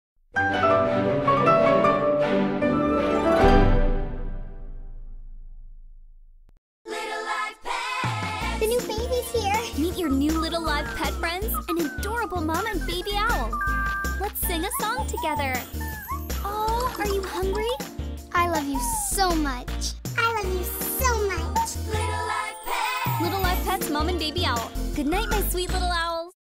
Little Live Pets (Fun Interactive Owls by Character) - ToySeek
Deliver Joy with a Toy at ToySeek!

Mother and baby owl love to care and share, with your help they make the perfect pair! Help mum feed and care for baby owl, listen as they happily sing tunes and hoot to each other. Baby Owl loves to be held and responds to your touch with happy sounds. Mother owl will record your voice and repeat back to you.